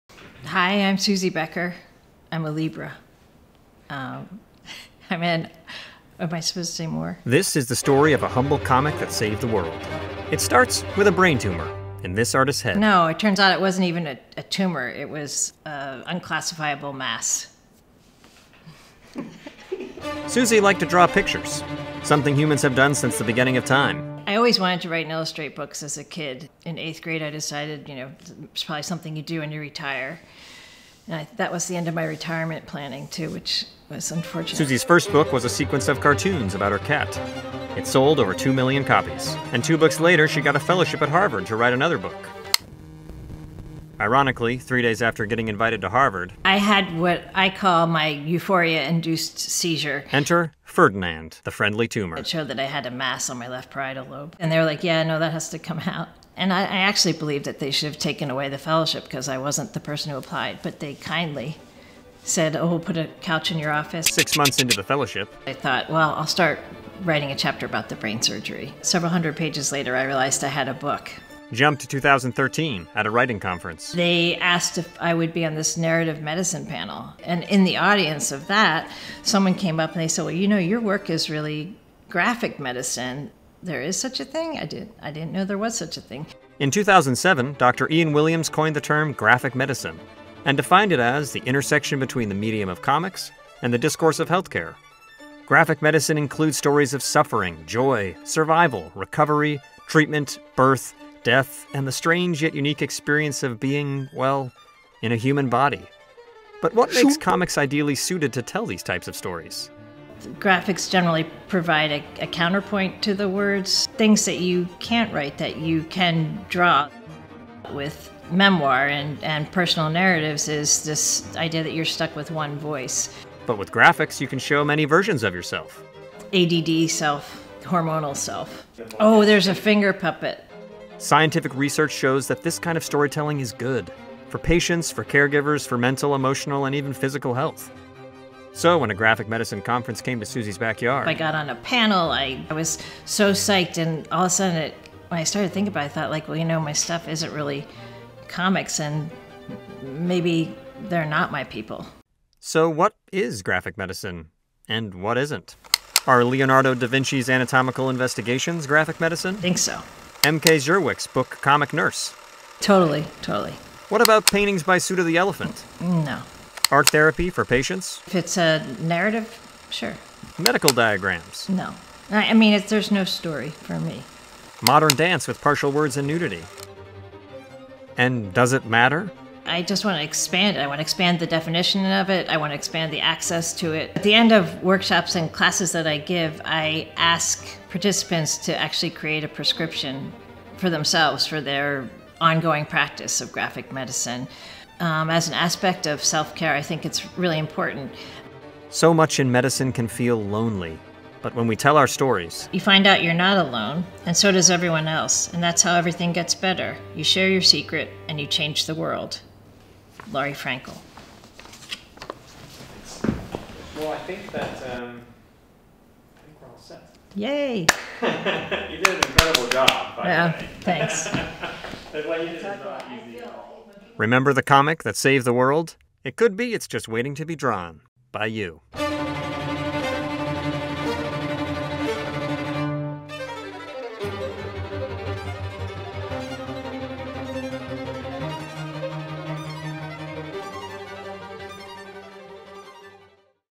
Suzy Becker Graphic Medicine
Suzy Becker is an author/illustrator and a New Yorker cartoonist. She  began her career as an award-winning advertising copywriter, then founded The Widget Factory, a greeting card company. Her first book was the internationally bestselling All I Need to Know I Learned from My Cat, now in the Double-Platinum edition All I Need to Know I Learned from My Cat (And Then Some). She has  since written and illustrated nine other books for kids and grown ups, including the graphic medicine memoirs I Had Brain Surgery, What’s Your Excuse? and One Good Egg.